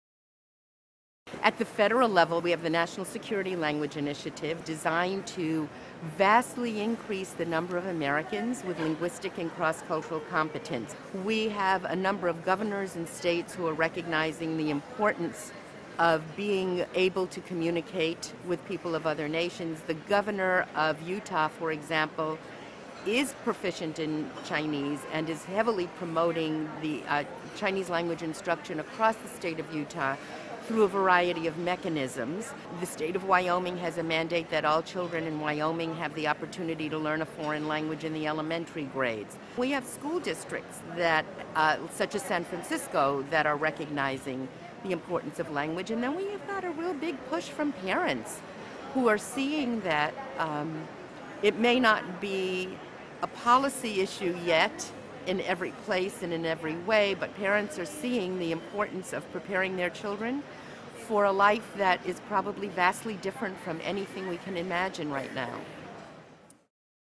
The United States of Language
Educational consultant from the National Center for Foreign Language, Mimi Met, explores how language is no longer a border issue.